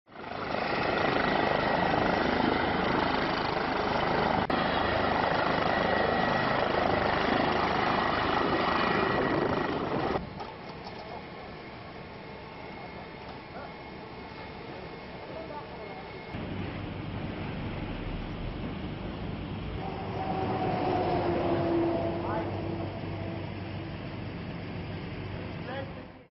Family killed as tanker explodes in Italy motorway crash
Six people including two children were killed in an explosion on a motorway in northern Italy, when a family car was shunted into a petrol tanker, according to police and media reports.